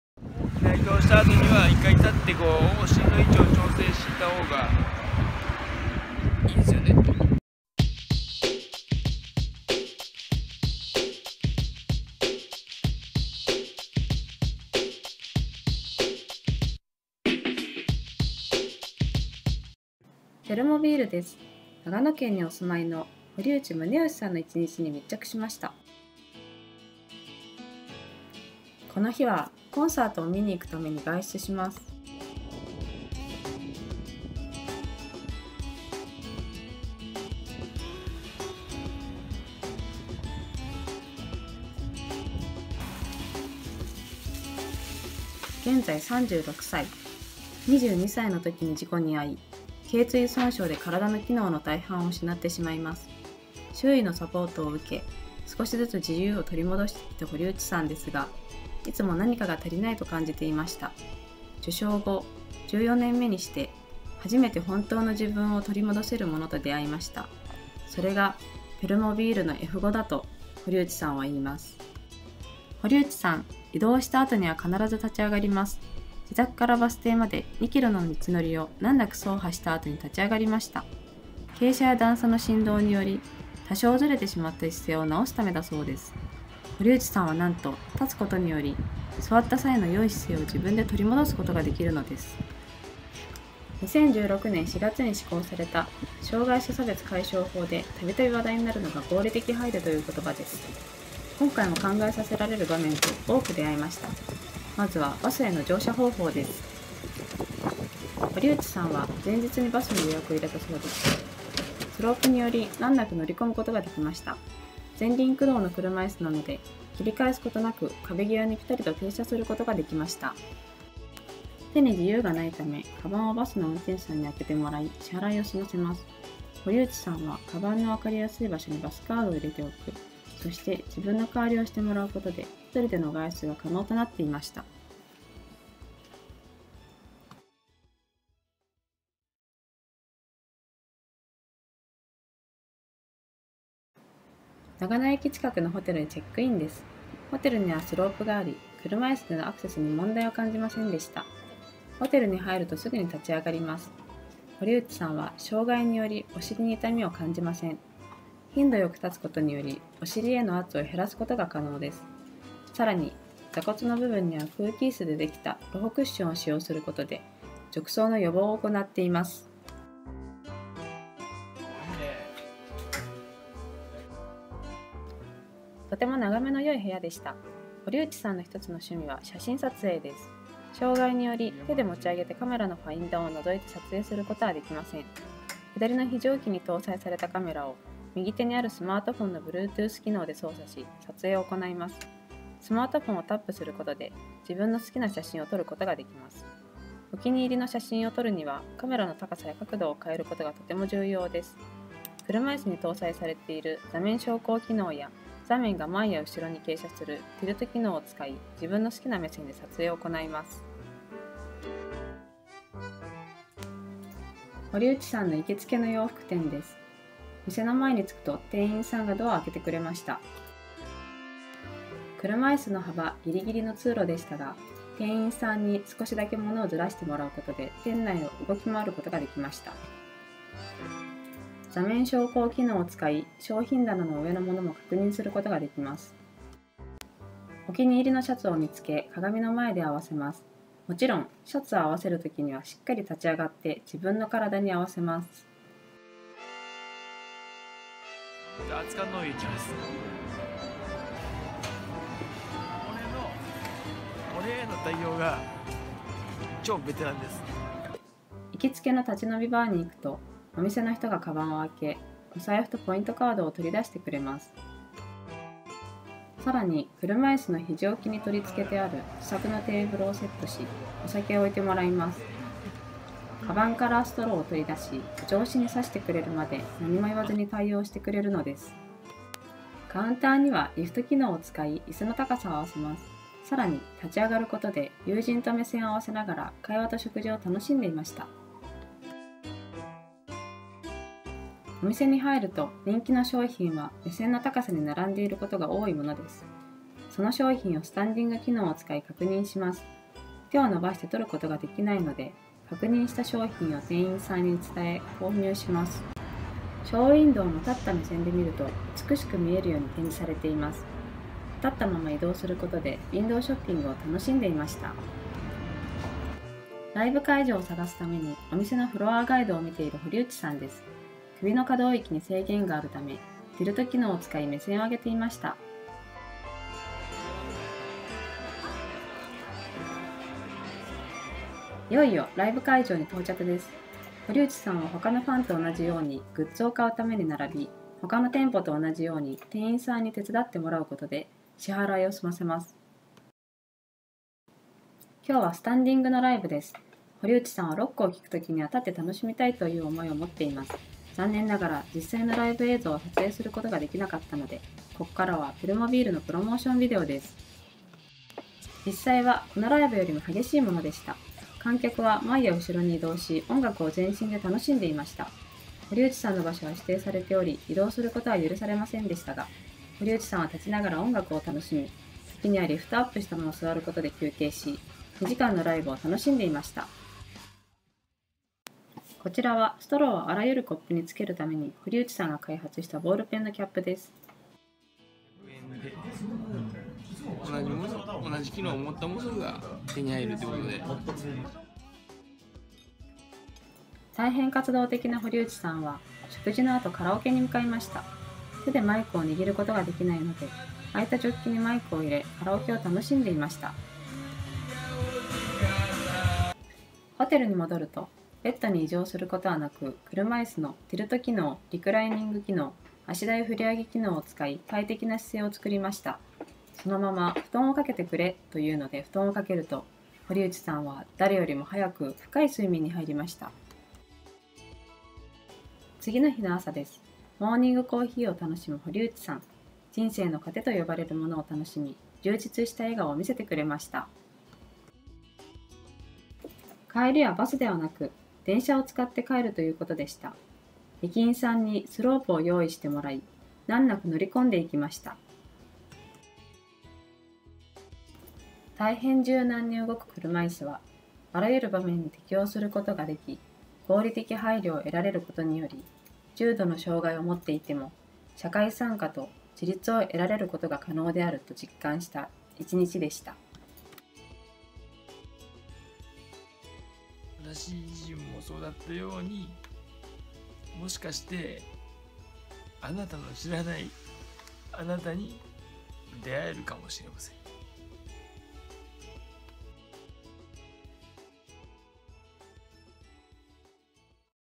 F5 Corpus VS -Muneyoshi Horiuchi- Permobil Japan
Permobil F5CorpusVS ペルモビールの電動車いすの立ち上がり機能を実際の生活で利用する堀内宗喜さんの1日に密着しました。柔軟に姿勢を変え移動が可能な電動車いすがいかに生活に影響を与えるのかご覧いただけます。